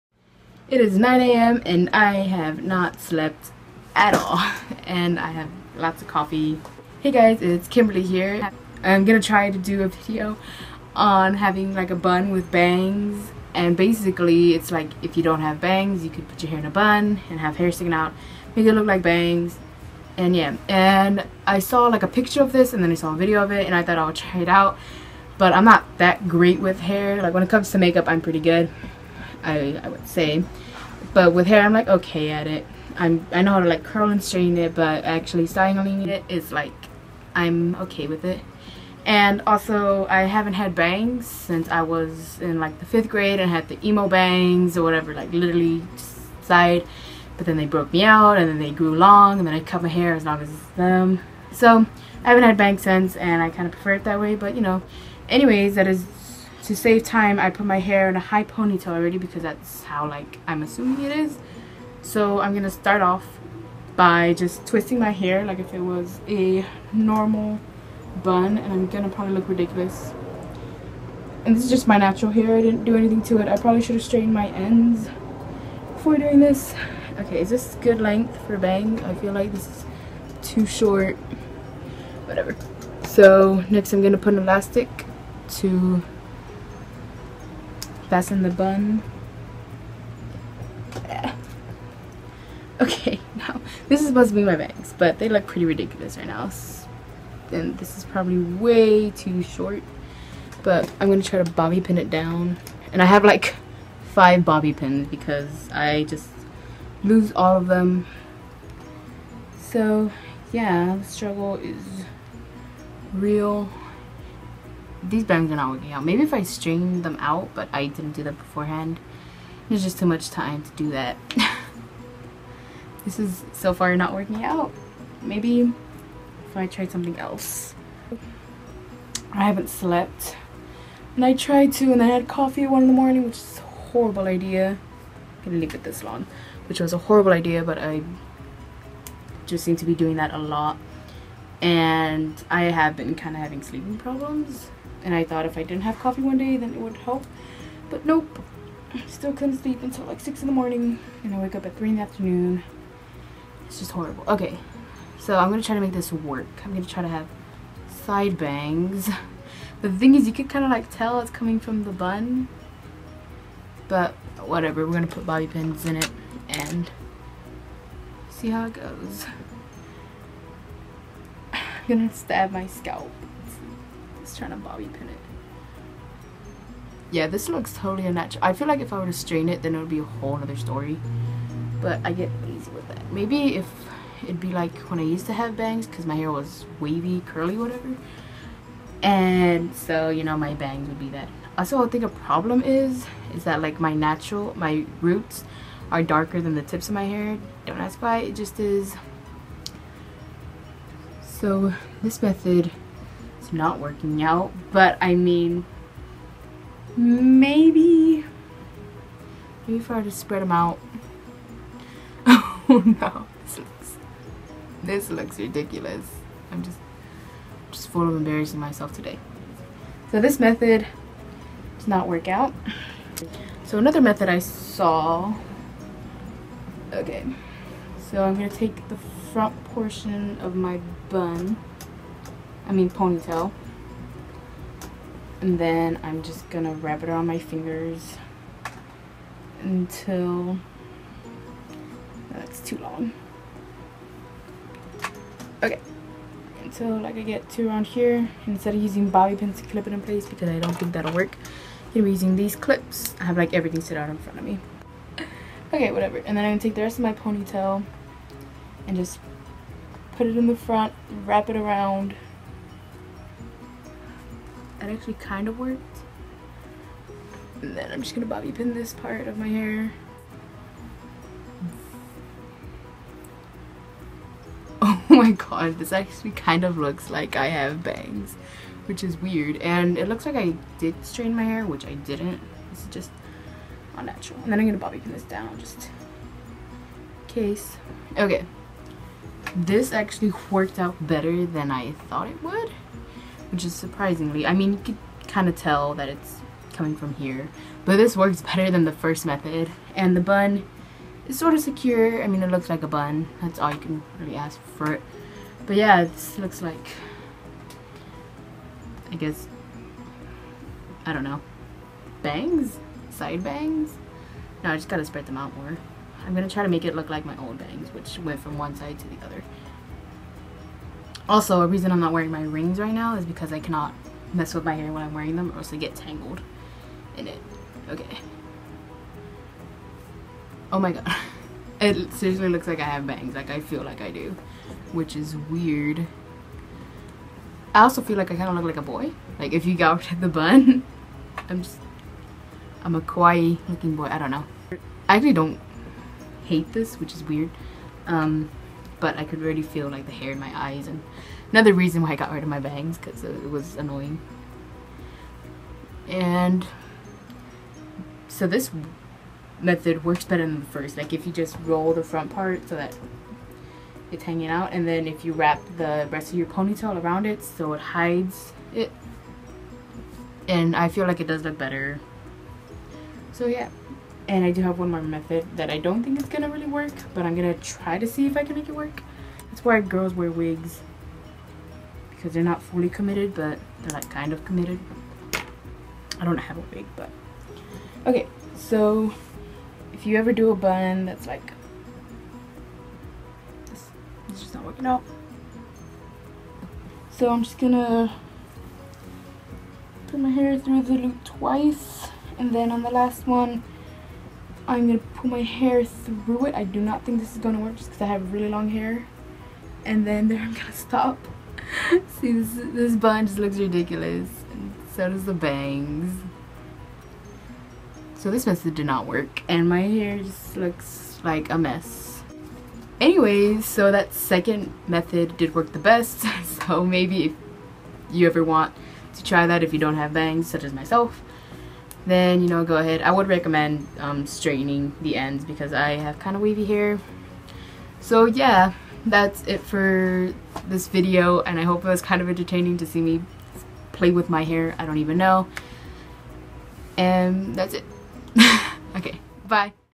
Trying Bun With Bangs
Hey guys! So for today's video I wanted to try something that I've seen around and I first got inspiration from when I saw a photo a few weeks ago. I hope it's funny and not awkward :D also please don't point out my horrible eyebrows that I didn't fill in because I haven't gotten them done while I was filming this. Anyways enjoys!